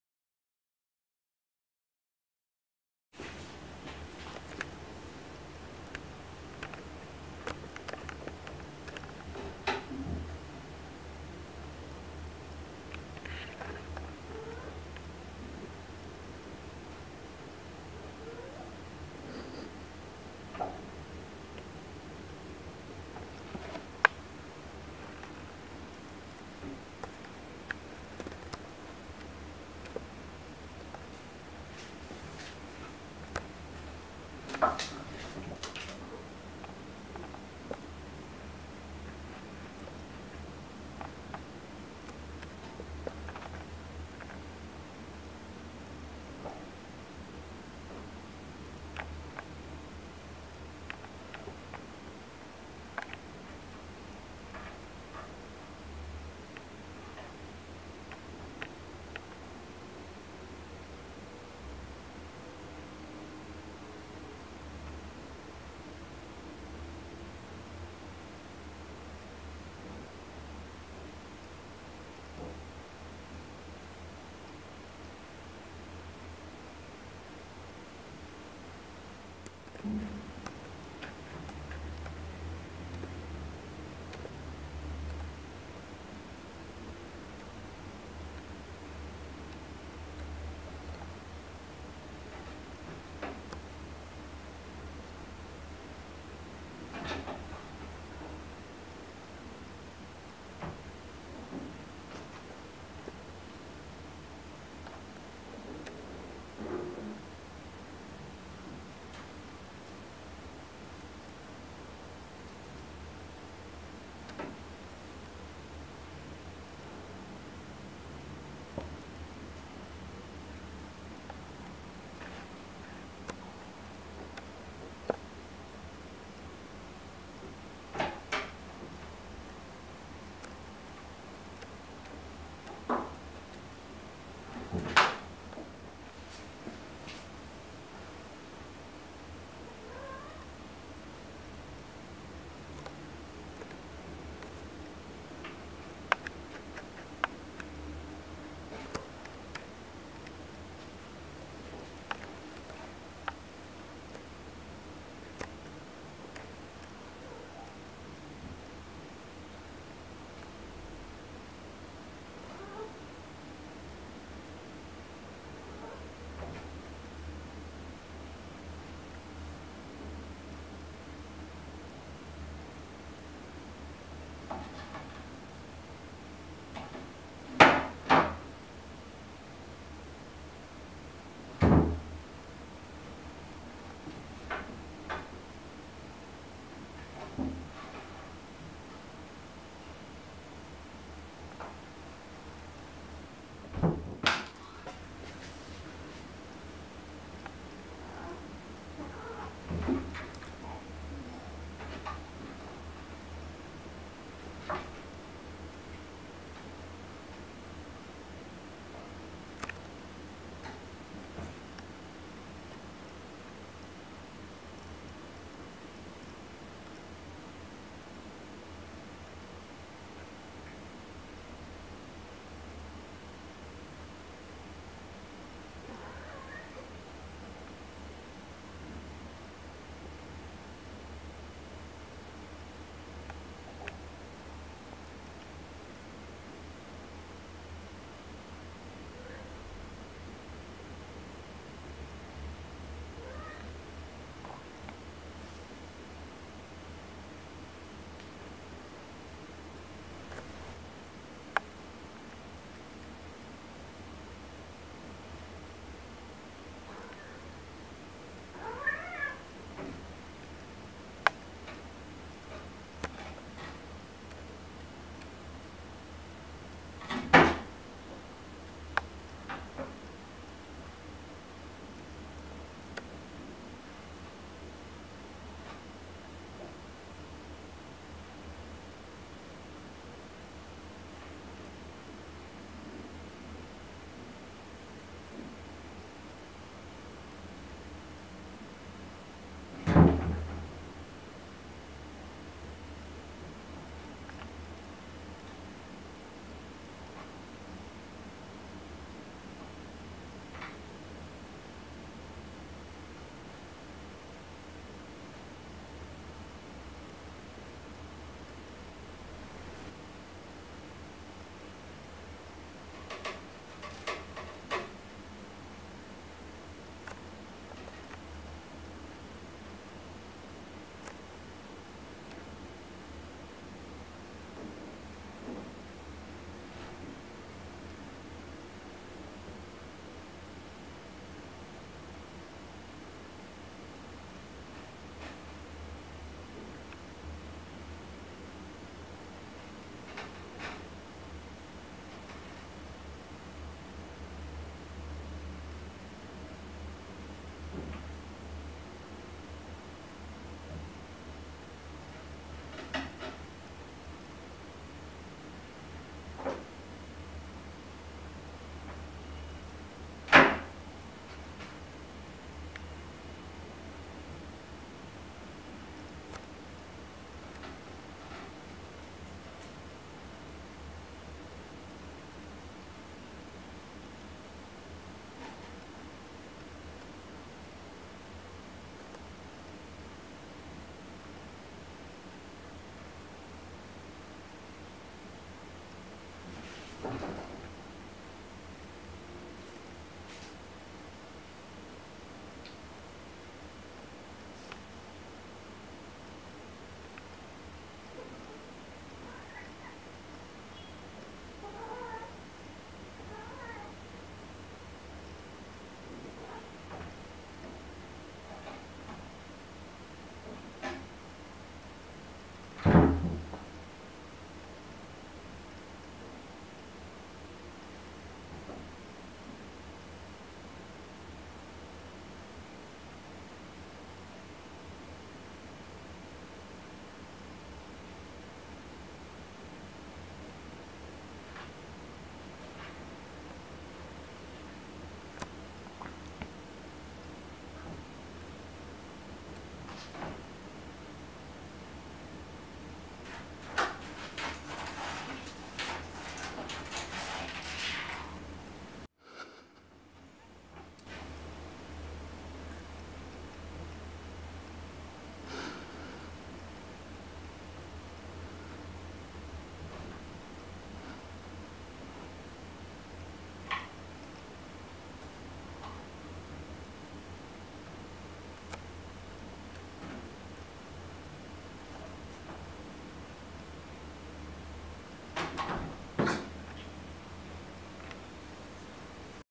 キャットタワー初体験 -Cat Climber-
hime最鍾意嘅蝦仔喺個過新Cat tree嘅頂。
為咗要救返蝦仔，佢不停自問自答係咁講嘢。

4:14 好鍾意佢扭呀扭
7:23 仲做埋引體上升